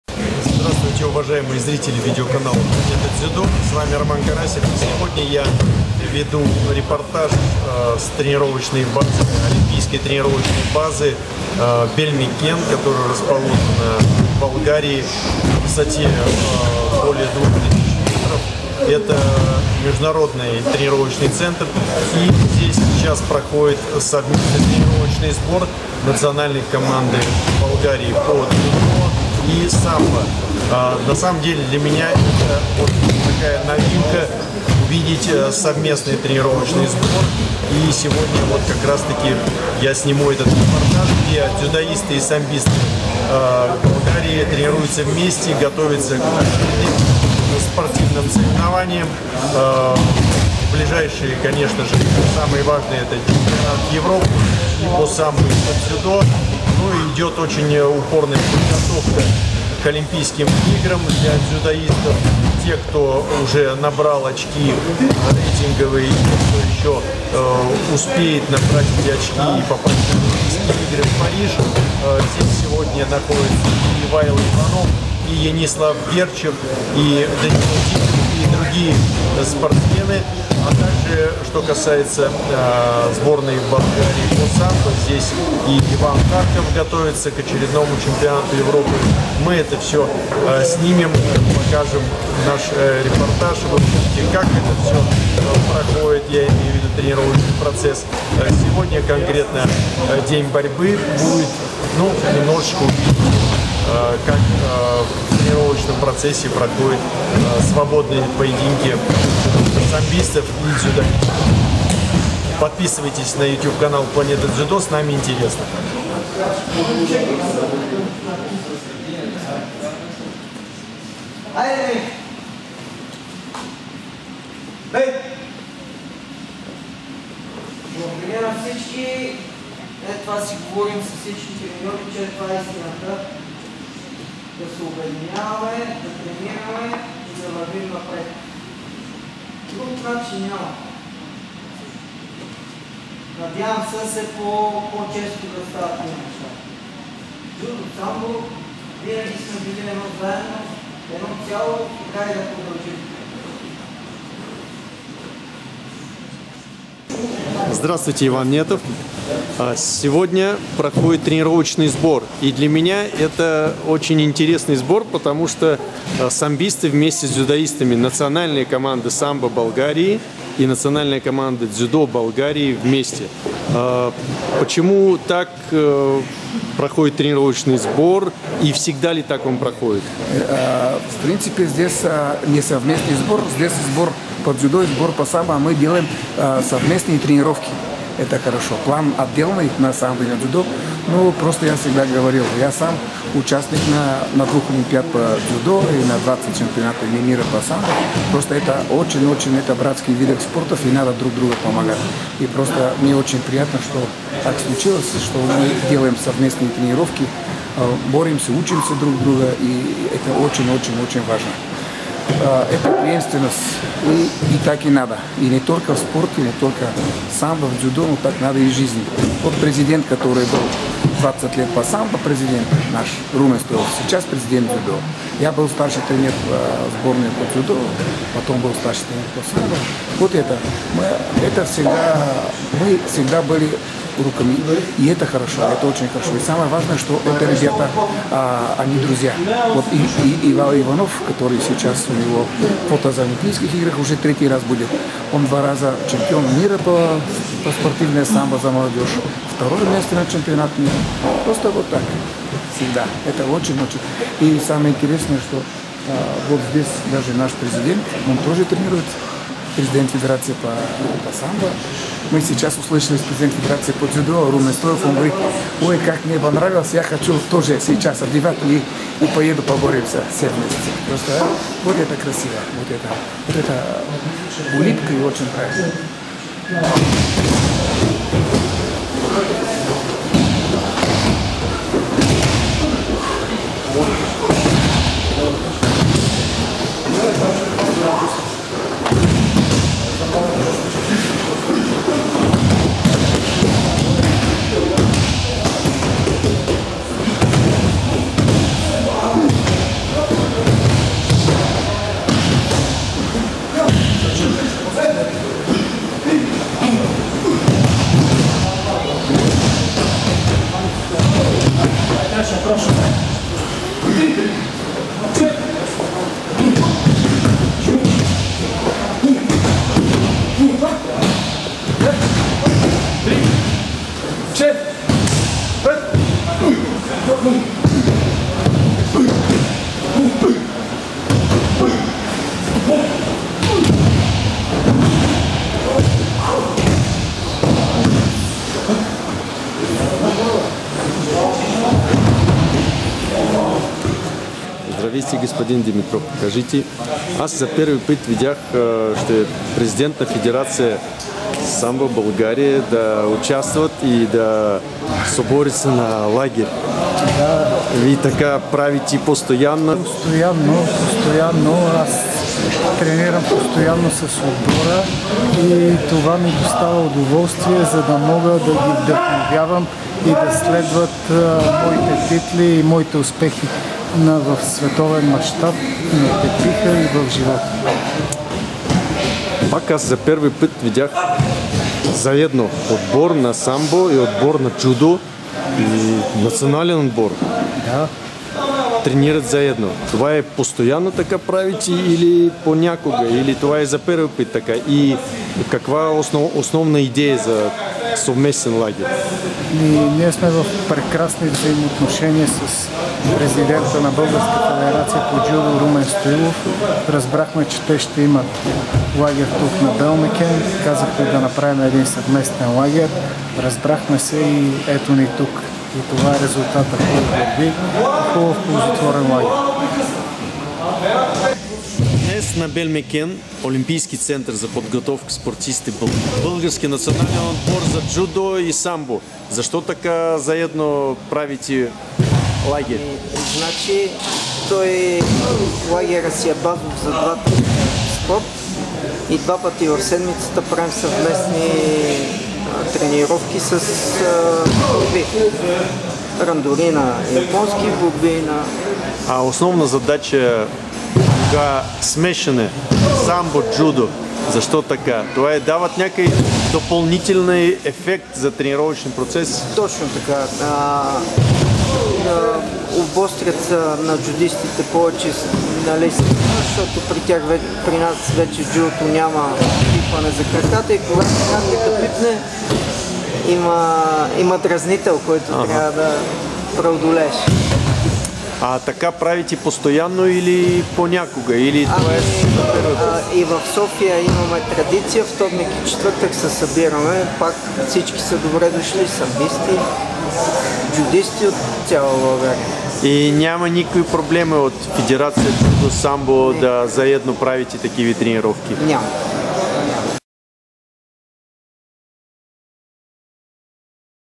Тренировочный лагерь сборной Болгарии по САМБО и ДЗЮДО Олимпийский центр подготовки Бельмекен
Специальный репортаж Романа Карасева из Болгарии о подготовке сборных команд Болгарии по ДЗЮДО и САМБО на высокогорной олимпийской спортивной базе Бельмекен.
🡺 Купить кимоно дзюдо, пояса дзюдо, сувениры дзюдо Fighting Films в России
🡺Купете кимона за джудо, колани за джудо, сувенири за джудо от Fighting Films в България